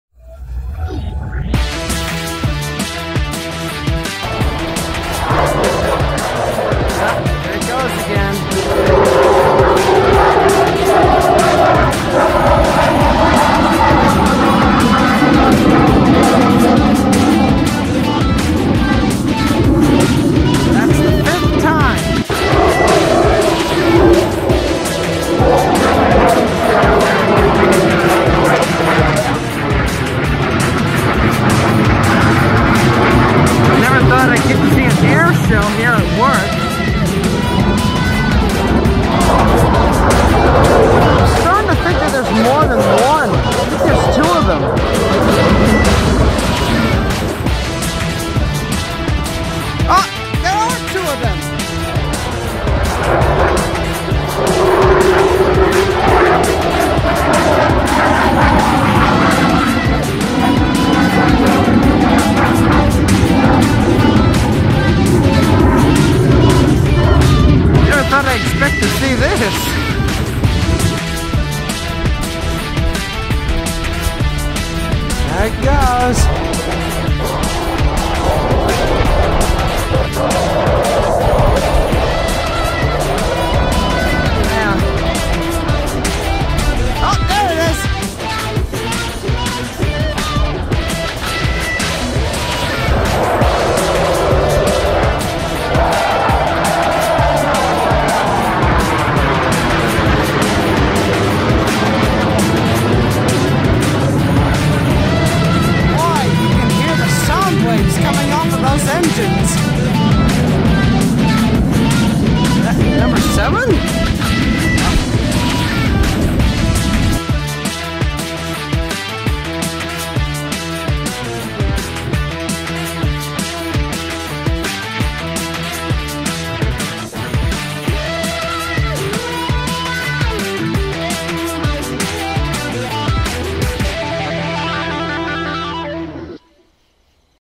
the crowd spotted a couple of jets flying over the store.
I don't know why, but all of a sudden, the crowd spotted a few military jets flying over our store they were circling us.